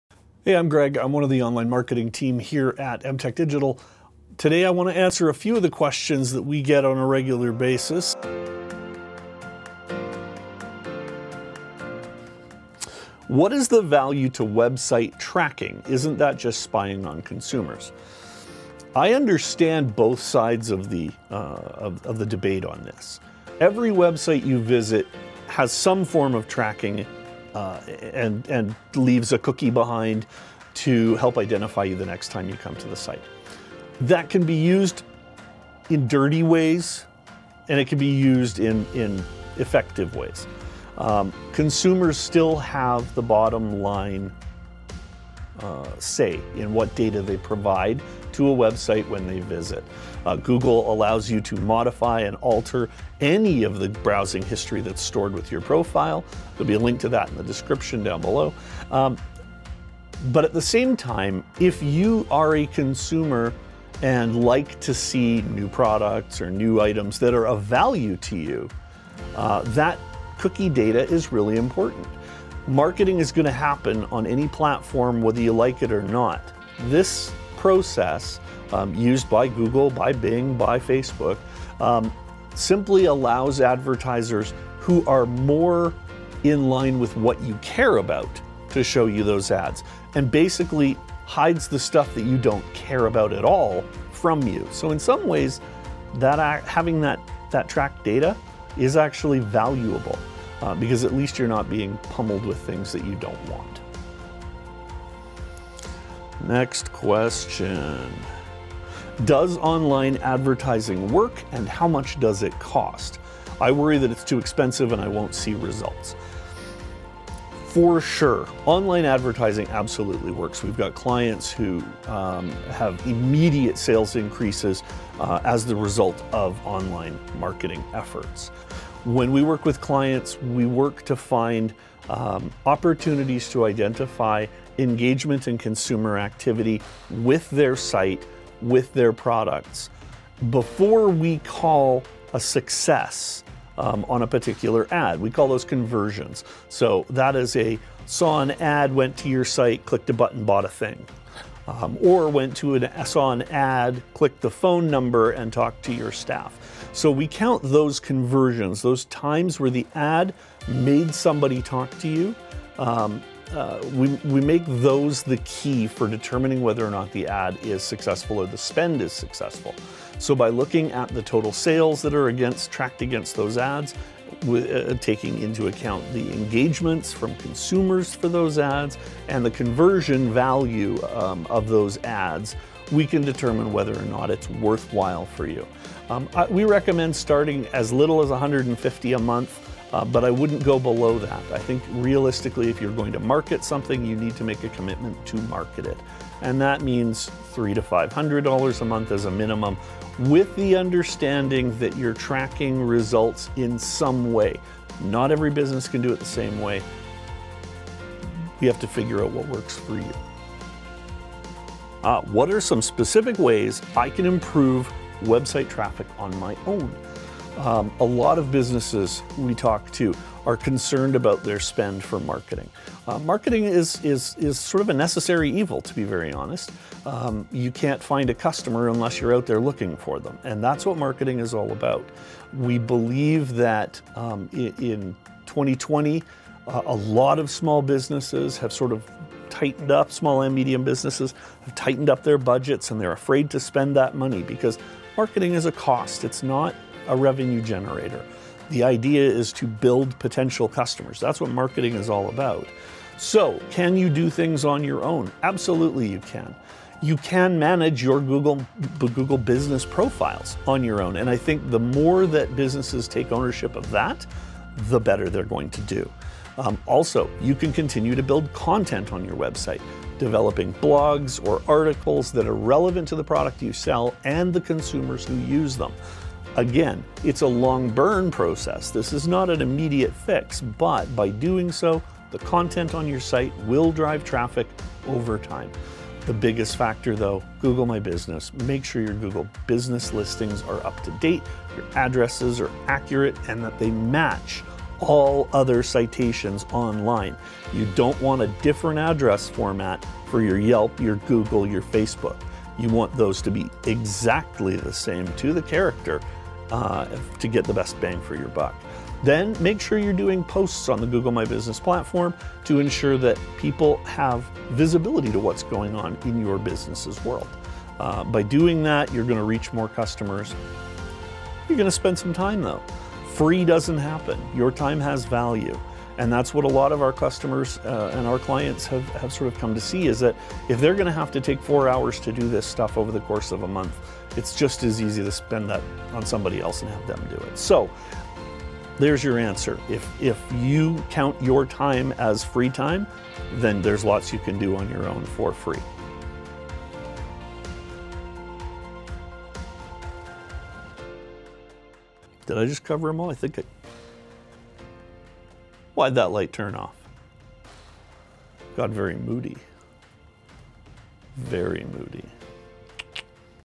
Answering Your Online Marketing Questions - Part 2
Mtek Digital gets questions every day about online marketing.  Occasionally it's a question from the internet, or a question during a client conversation but every time we talk about how to move marketing to the online space from 'traditional' print and magazine advertising - the questions come up.

If you have questions - be sure to put them in the comments, and we'll do our best to answer them in a future video!